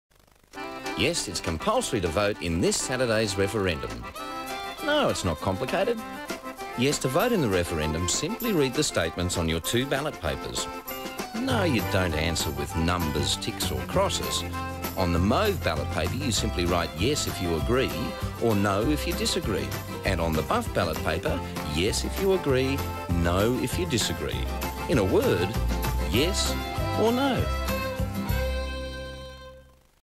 Australian Electoral Commission (Republic Referendum) - 1999 Australian TV Commercial
Recorded during a broadcast of the film "Michael" on ATN-7 Sydney, which was transmitted on 31 October 1999 at 8:30pm.